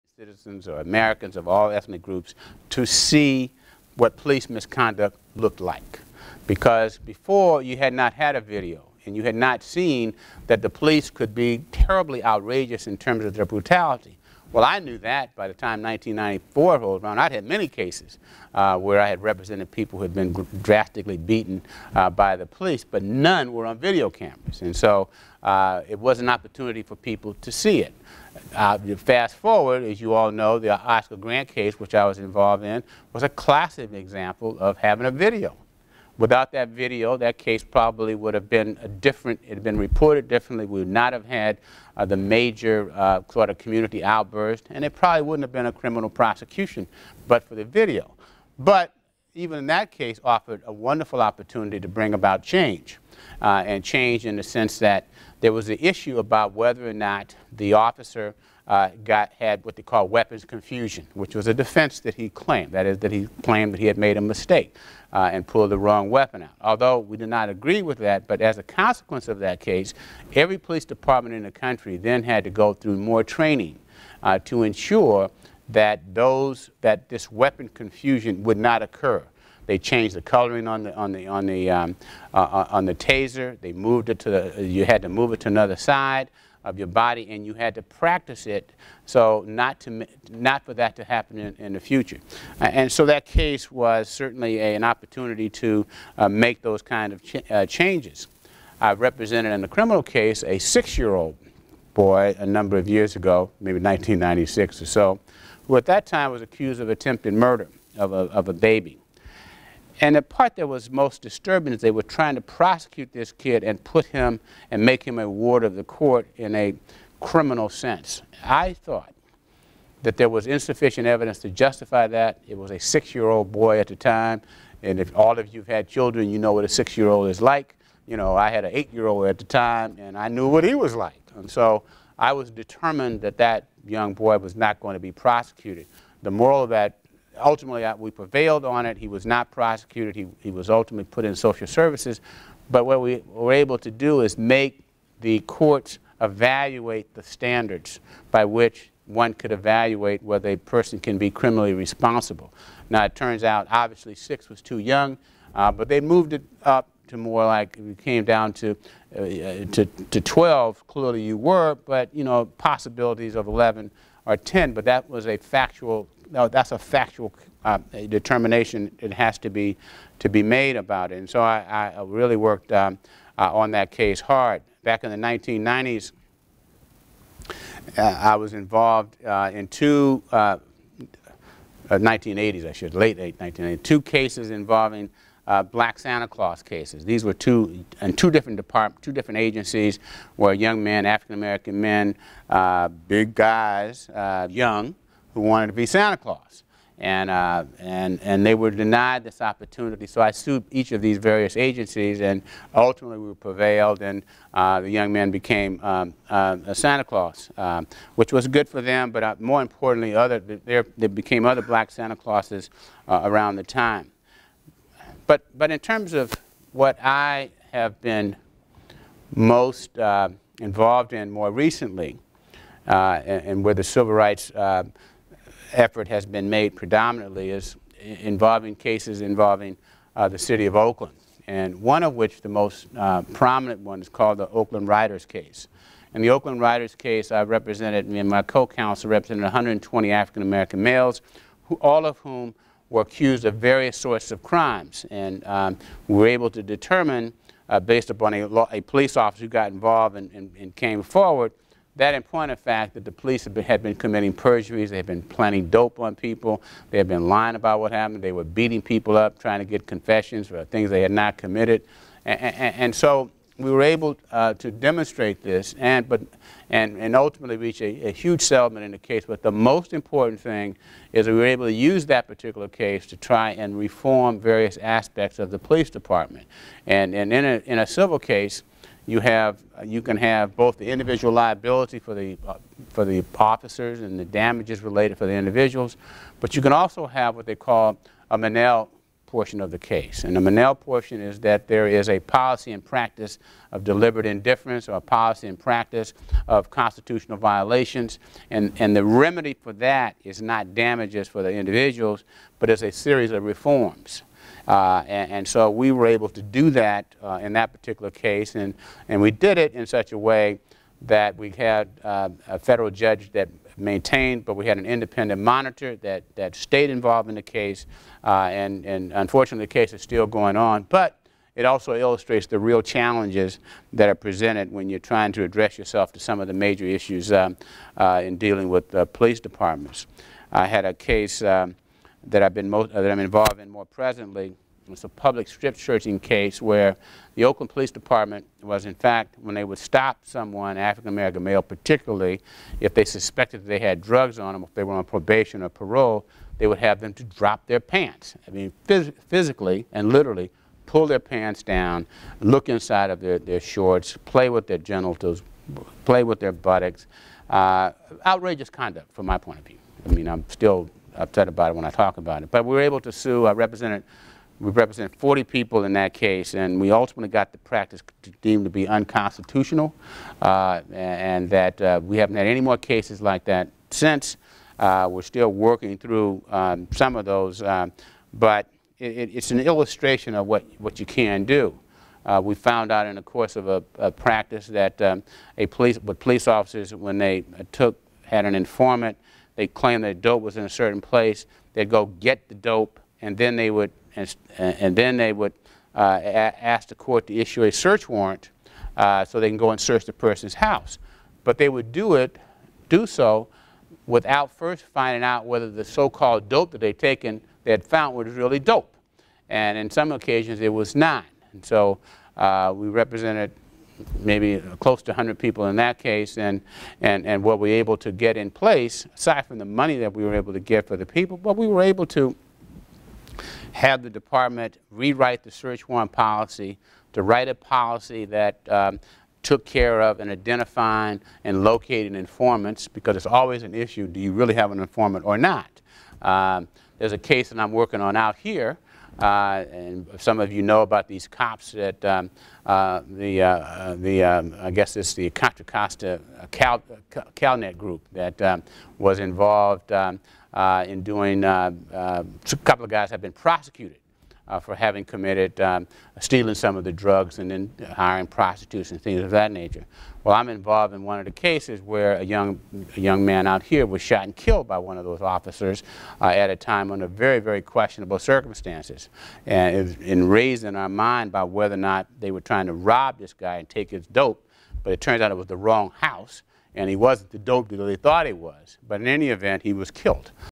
John Burris 2.mov
Part 2 hear from Attorney John L. Burris
speak on Civil Rights. He is best known for his work in the area
of plaintiff's civil rights over the last 25 plus years and as a legal
analyst on Fox, MSNBC, Court TV, CNN and many local
television and radio stations. His primary areas of focus for his
practice include cases involving police misconduct, employment
discrimination and criminal defense.